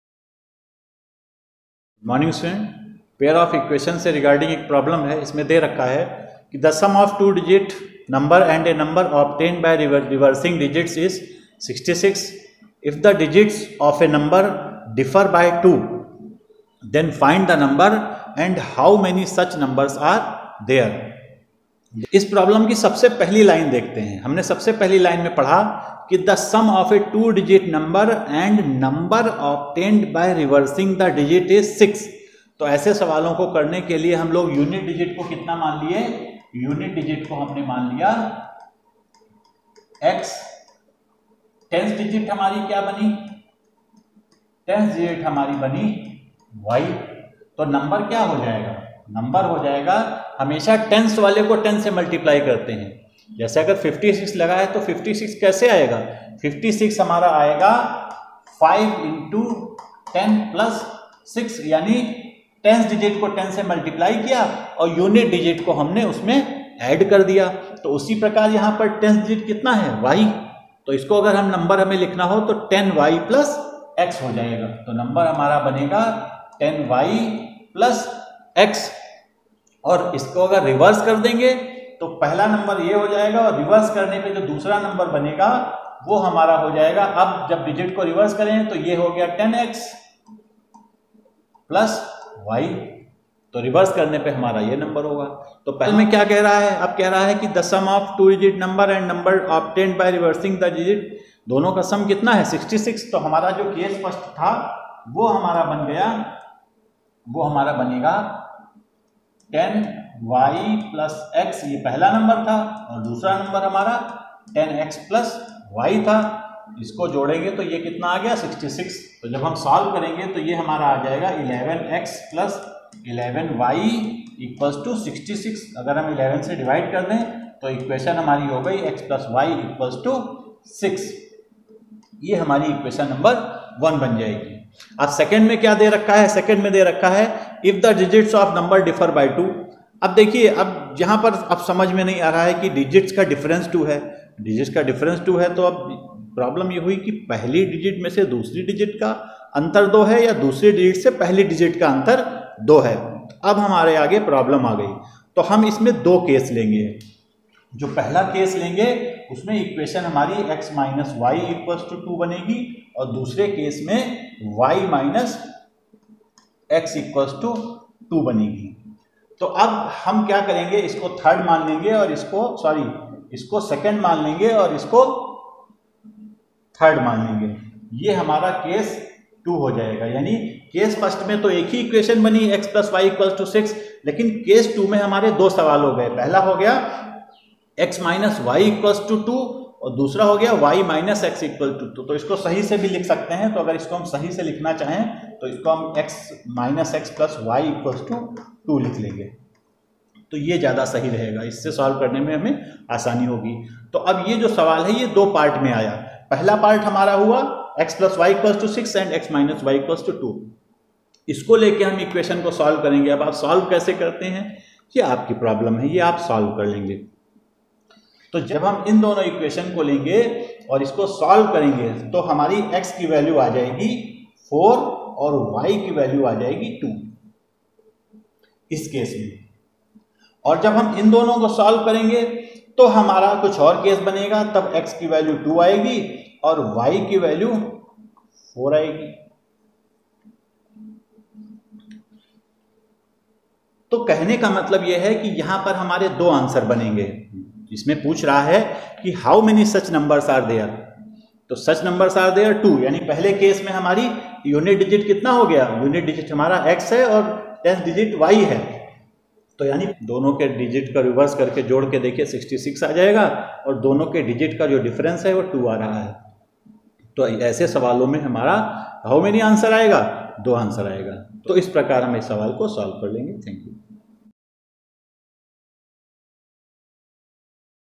The sum of a two digit number and the number obtained by reversing the digits is 66 | example 13
The sum of a two-digit number and the number obtained by reversing the digits is 66. If the digits of the number differ by 2, find the number. How many such numbers are there?
The sum of a two-digit number and the number obtained by reversing the digits is 66. If the digits of the number differ by 2, find the number.
Example 13
Chapter 3
Pair of linear equations in two variables
Class 10